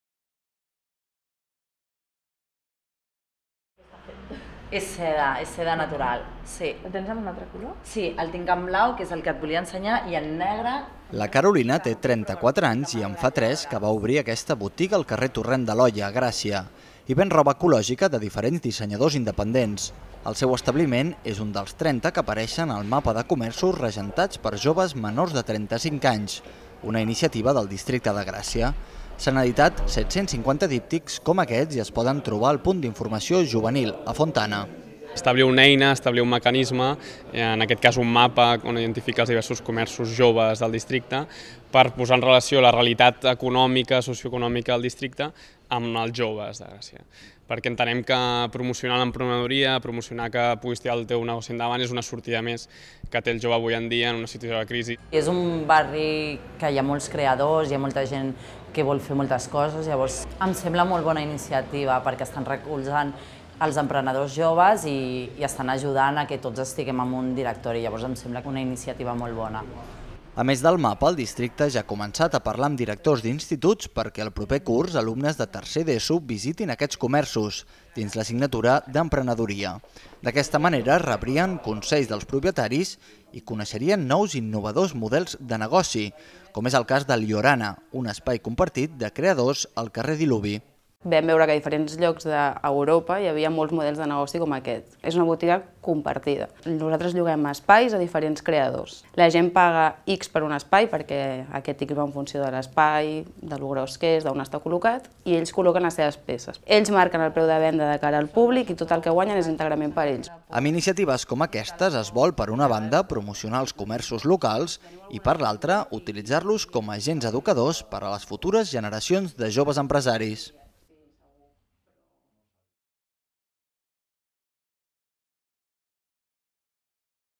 Un barri de joves emprenedors
El Districte de Gràcia ha impulsat un mapa de situació dels comerços regentats per joves menors de 35 anys. Més informació en aquest enllaç